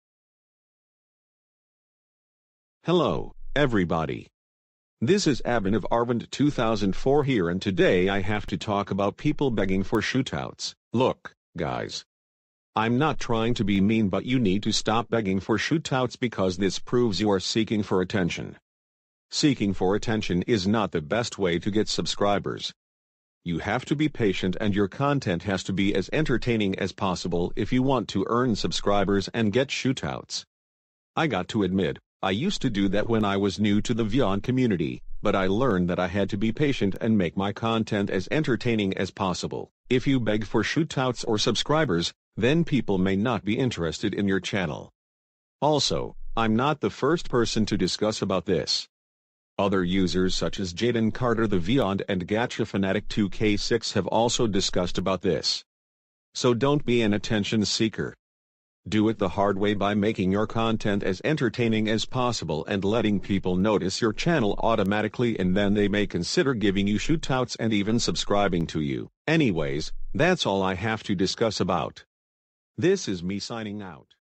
Talking about people who are begging for shoutouts
This is a very important video, so please watch!